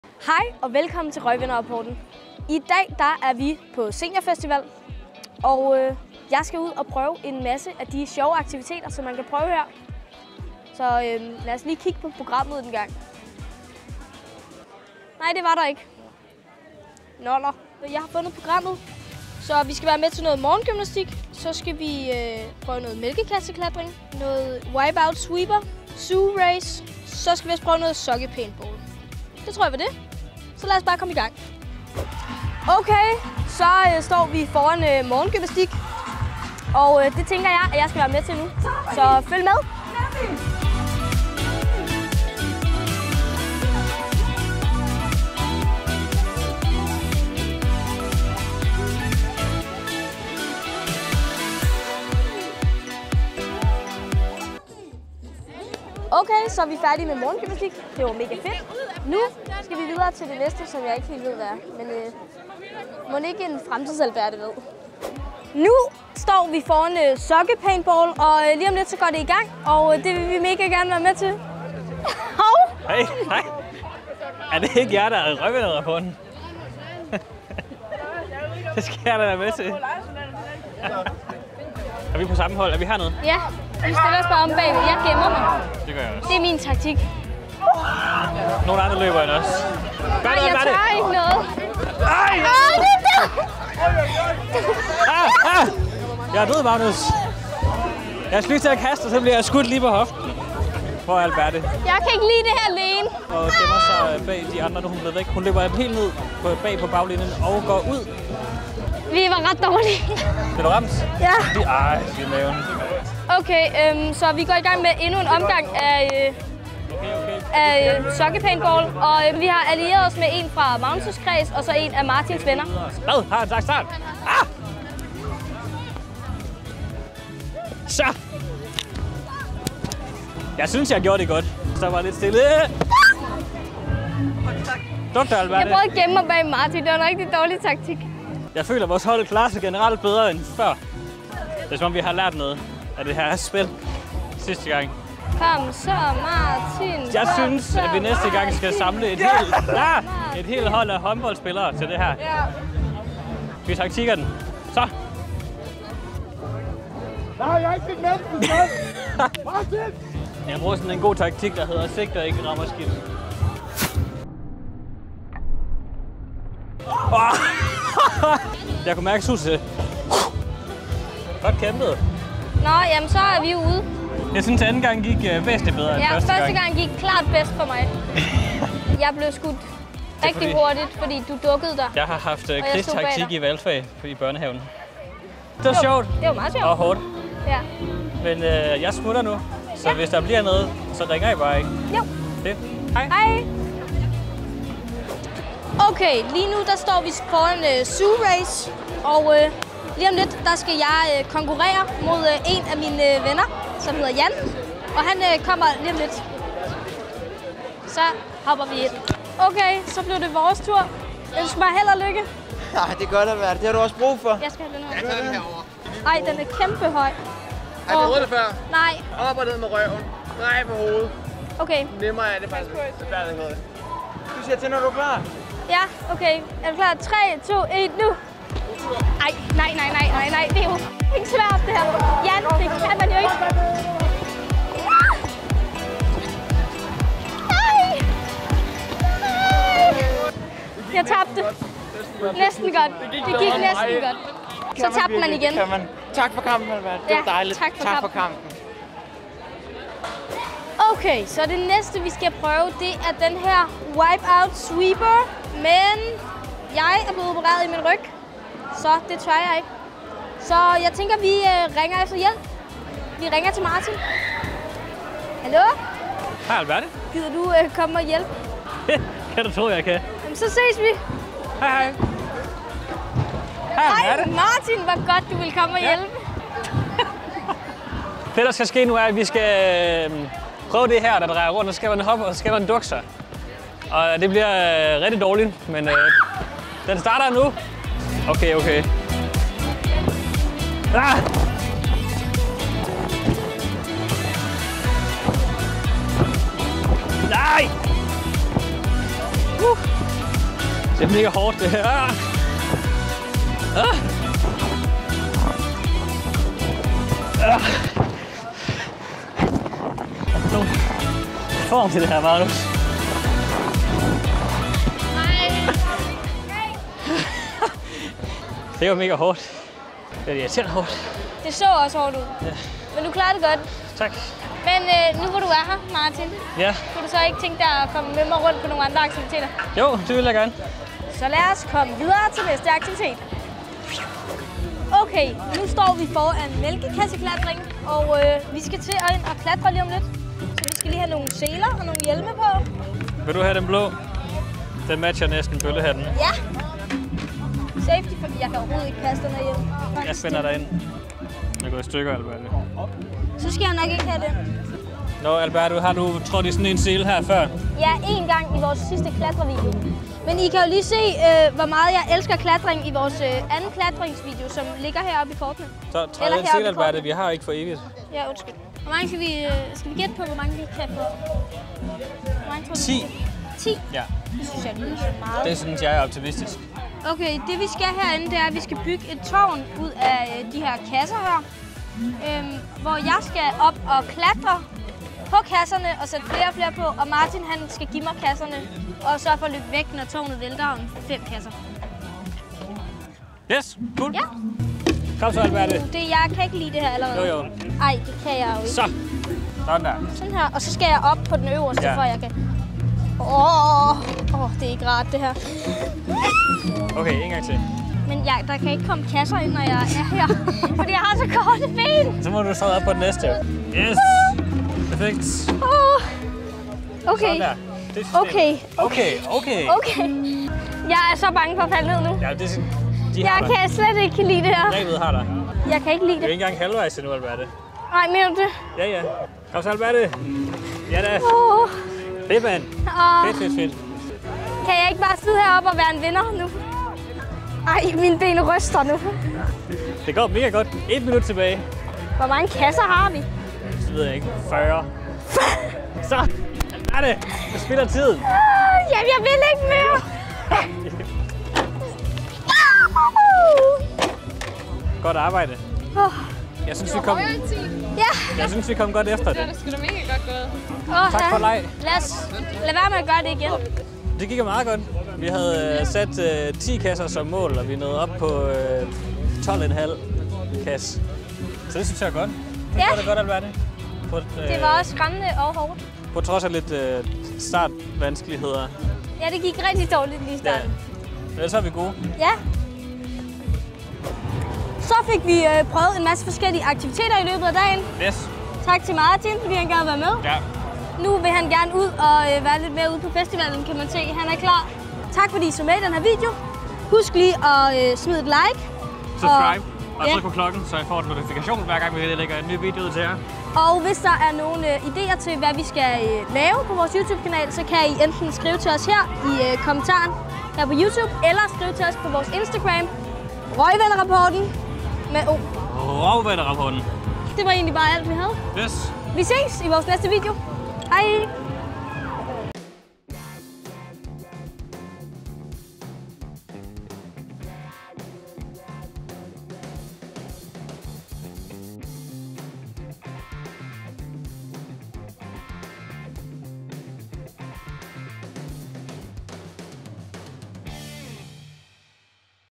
SENIORFESTIVAL Men Kun Aktiviteterne! | FDF Seniorfestival 2022
Vi har været på Seniorfestival! Alberte (og Martin) drog ud på de forskellige aktiviteter, som Seniorfestivalen havde at tilbyde.

VÆRTER – Alberte Jensen & Martin Hansen
KAMERA – Magnus Dahl